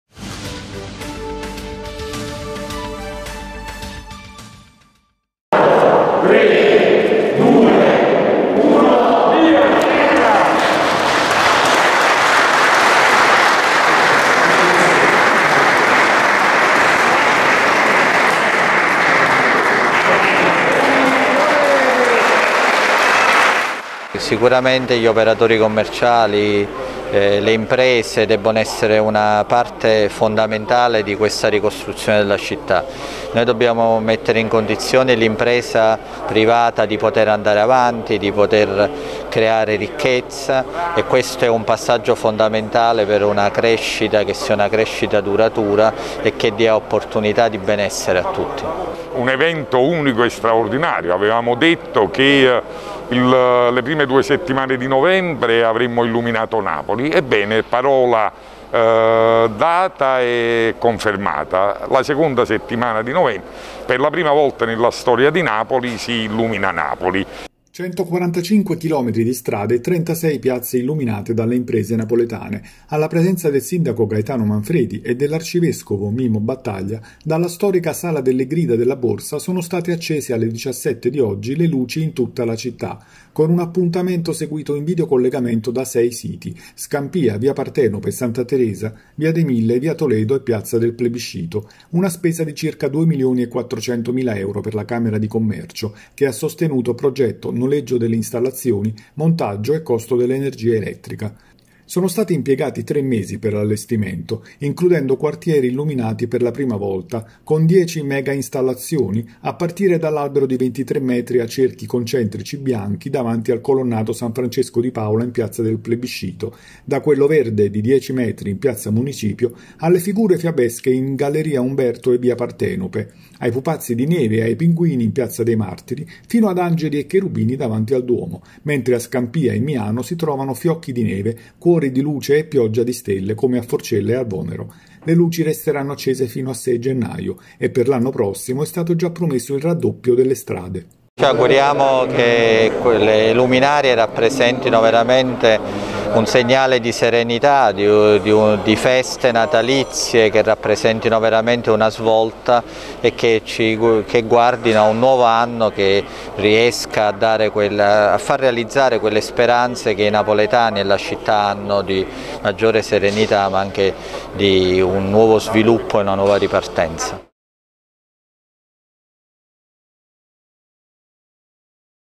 “Illuminiamo Napoli”: si accendono le luci in tutta la città
Il progetto della Camera di Commercio coinvolge 145 km di strade e 36 piazze: si sono accese le luci natalizie da Scampia al centro storico, fra mega installazioni, alberi, figure fiabesche, pupazzi e angeli. E per l'anno prossimo è stato già promesso il raddoppio delle strade.